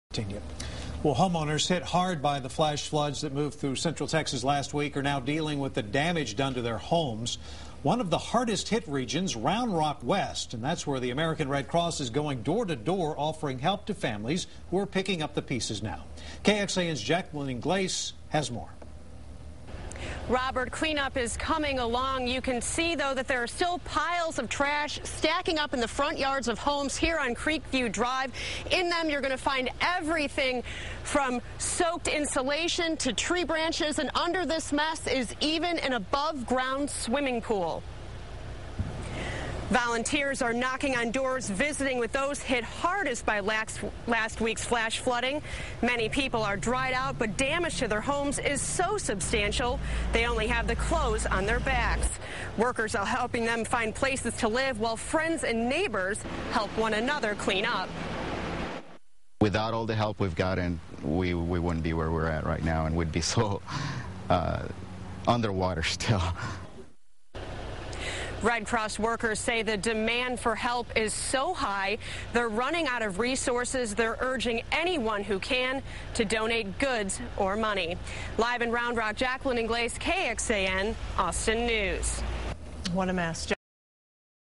KXAN-Flood victims deal with damage
Round Rock homeowners are not left dealing with the damage Tropical Storm Hermine left in its wake.